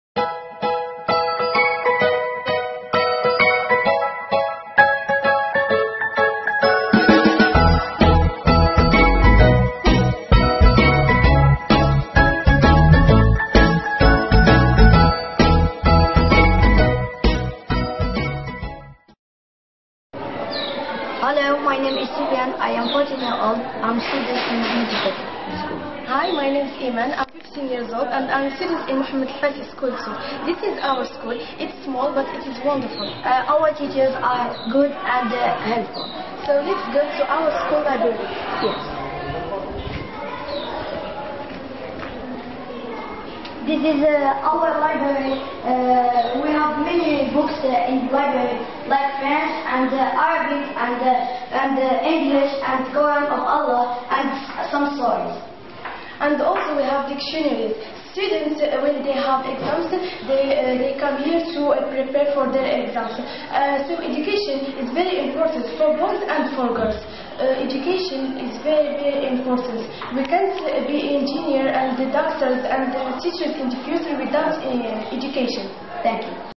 The importance of education by Mate circle Med El Fassi school students
My little kids, Imane and Soufiane talking about the importnace of education and reading. I'm always so proud of you little ones :)